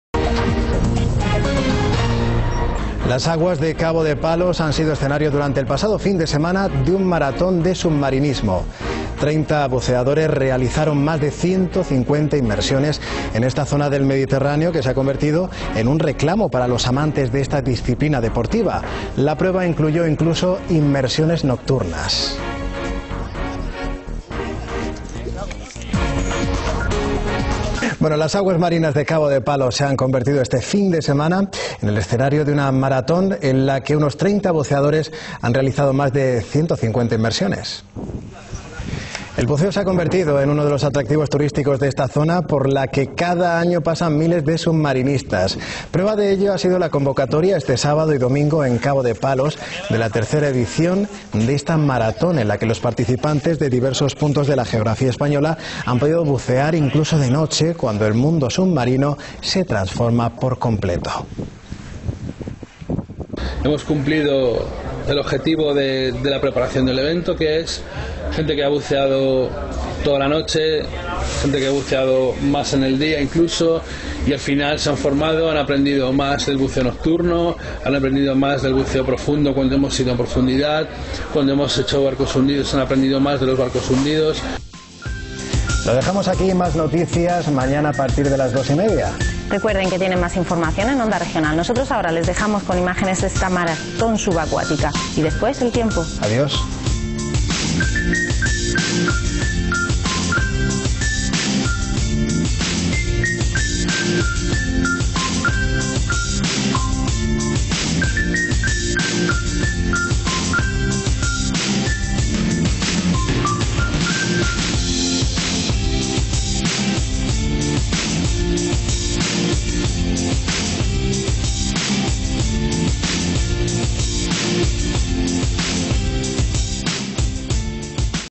III Scuba Diving Marathon Cabo de Palos
Am 25. und 26. Mai 2013 fand das Top Tauch Event, der III. Scuba Diving Marathon, in Cabo de Palos statt. Ein "Muss" für jeden aktiven Taucher, um die herrliche Unterwasserwelt im Süden von Spanien zu erleben!